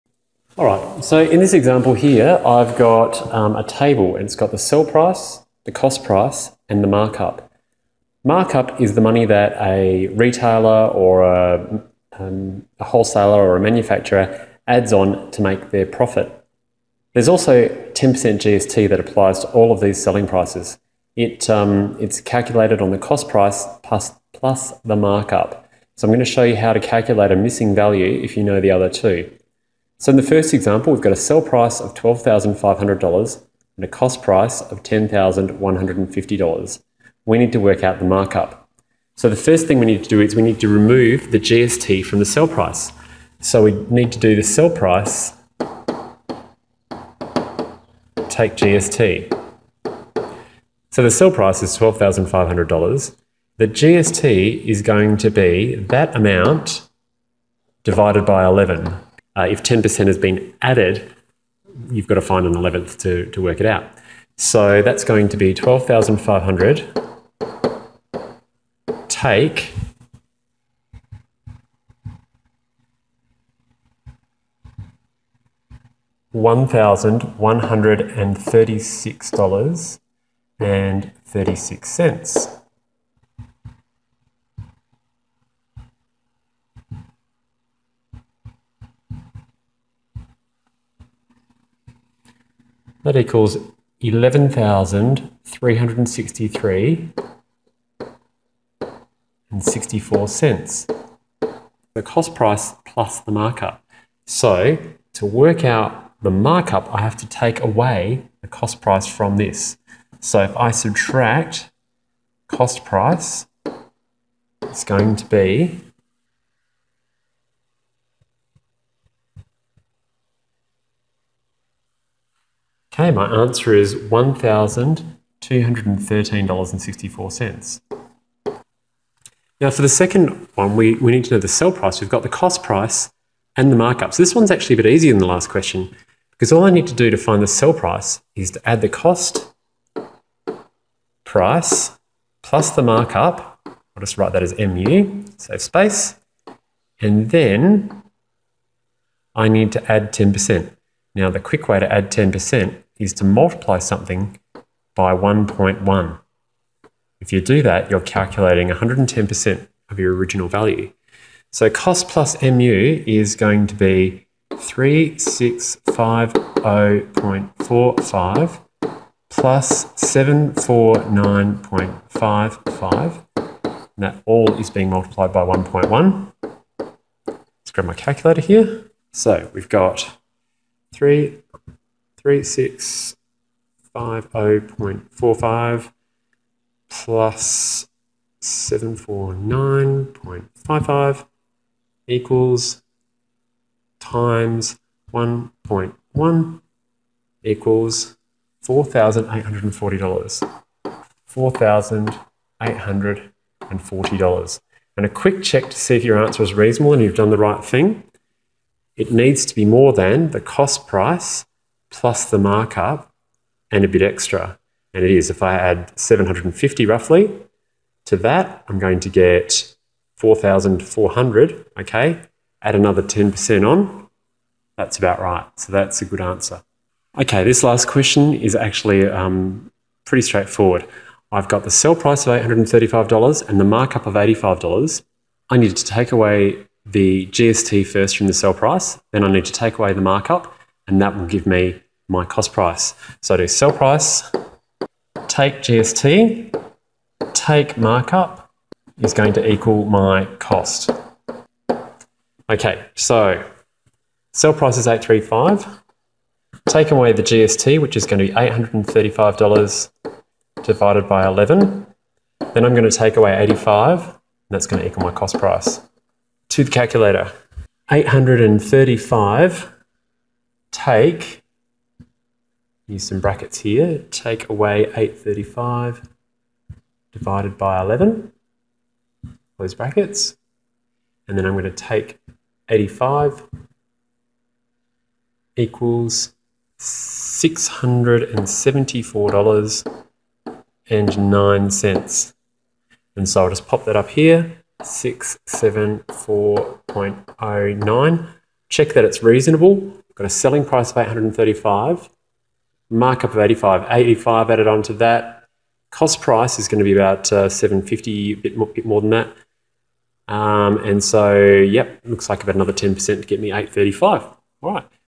Cost Price, Selling Price and Markup
Calculating one when you know the other two, including GST